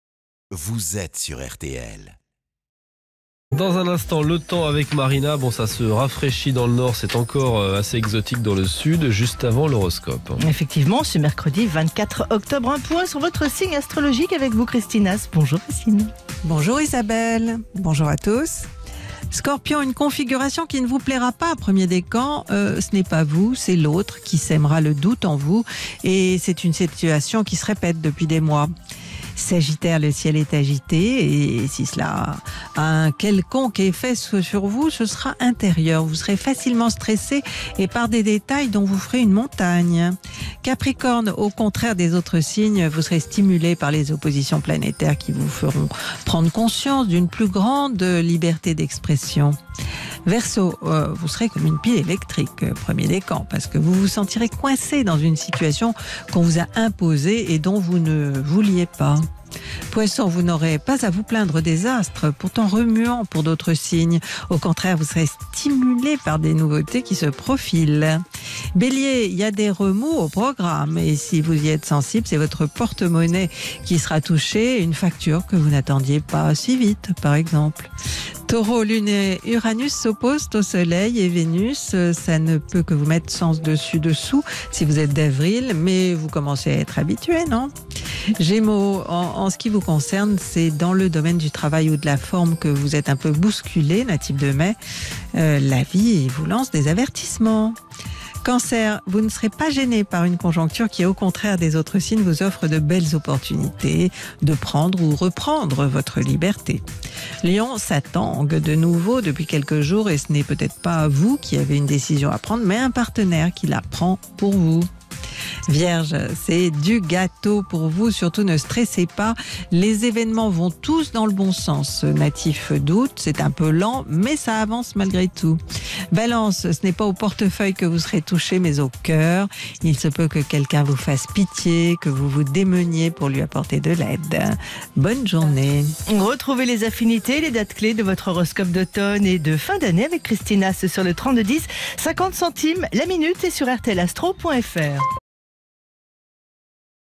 L'horoscope du 24 octobre 2018
Retrouvez L'horoscope du jour avec Christine Haas du 24 octobre 2018 sur RTL.fr.